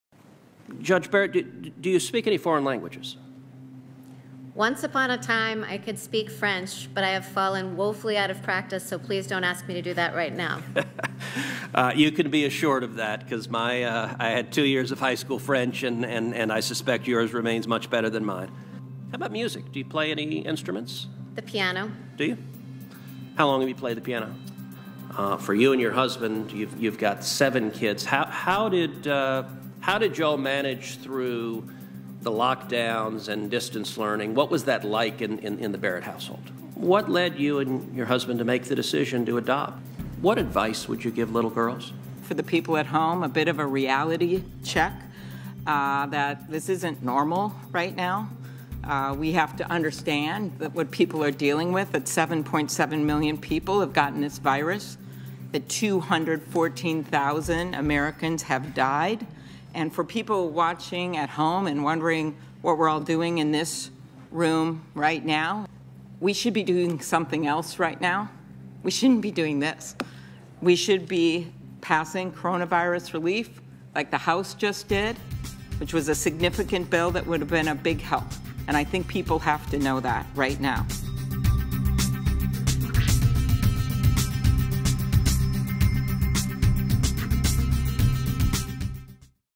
Senators Ted Cruz and Amy Klobuchar clash at Barrett hearing
Senators Ted Cruz and Amy Klobuchar sparred at the confirmation hearing for judge Amy Coney Barrett to the Supreme Court. After Cruz asked Barrett a series of personal questions, Klobuchar responded "this isn't normal."